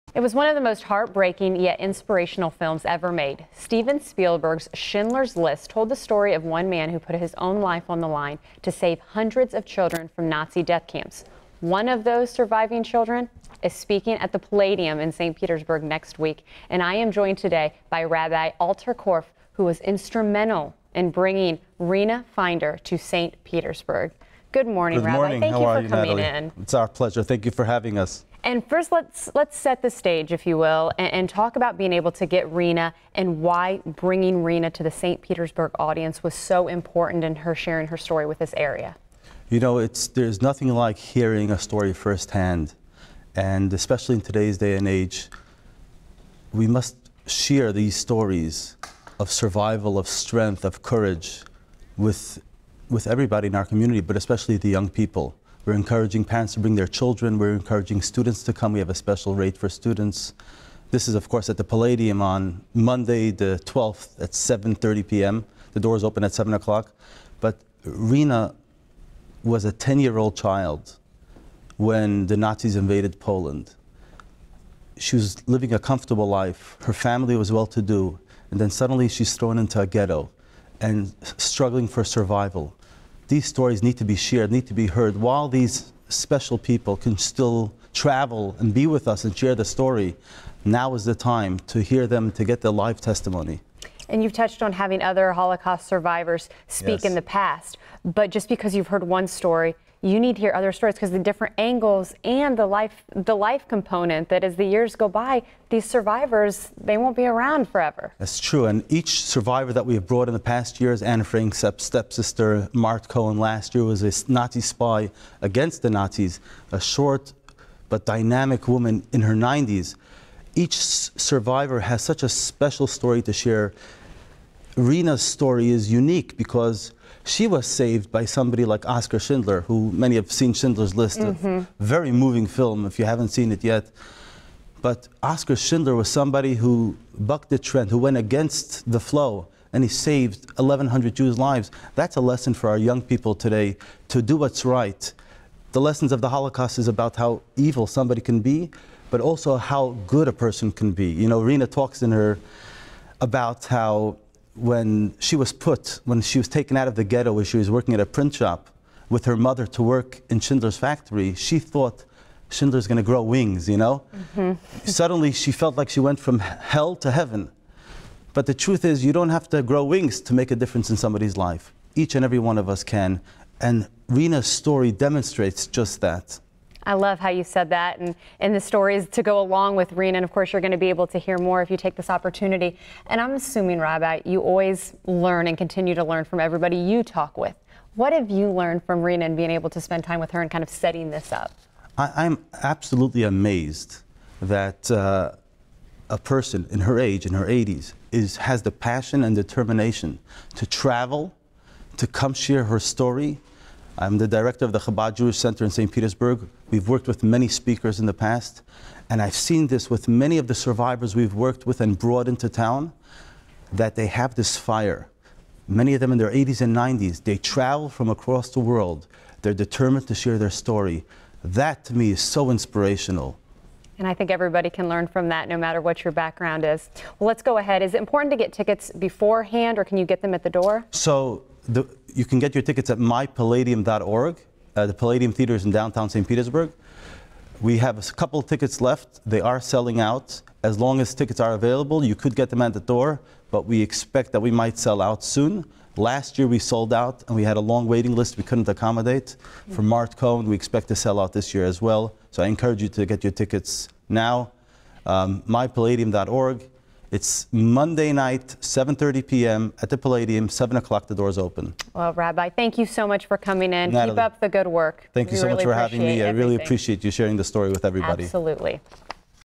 Schlinder's List Survivor
One of the last Schlinder's List survivors will be speaking in St. Petersburg.